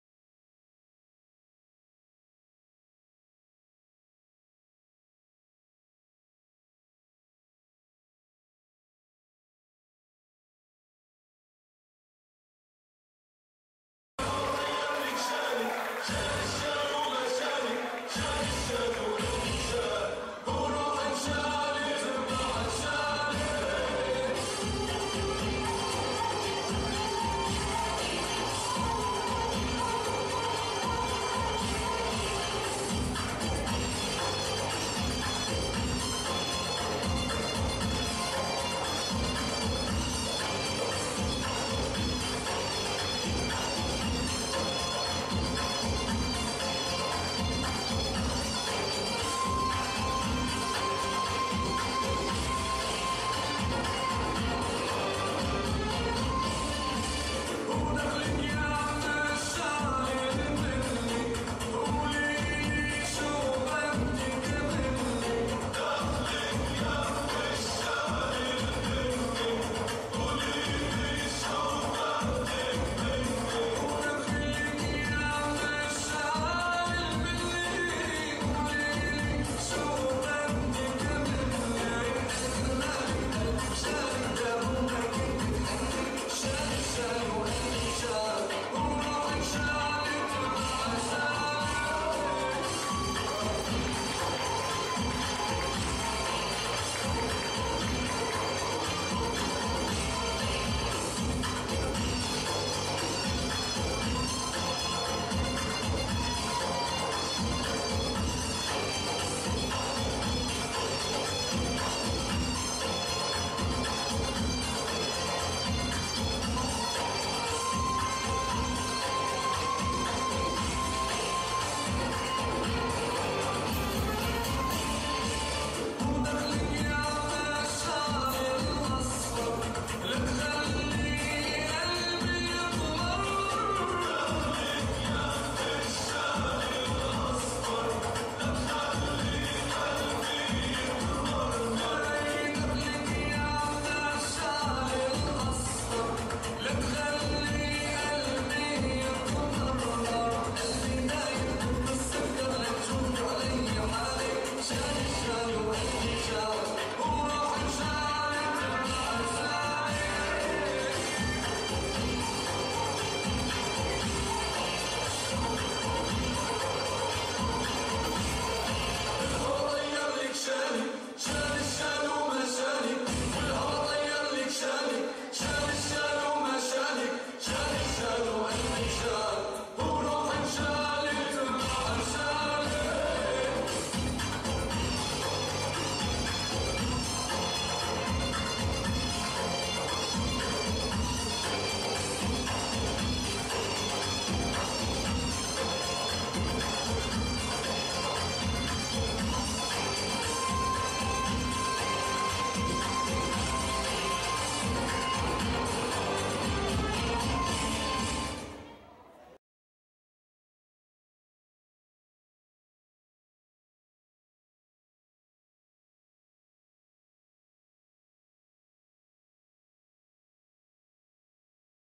Natan Lune | 1° EDAEC Bahia
1º Encontro de Dança Árabe em Euclides da Cunha/Ba
29 e 30 de Março/2014
Realização e Organização: Karina Macedo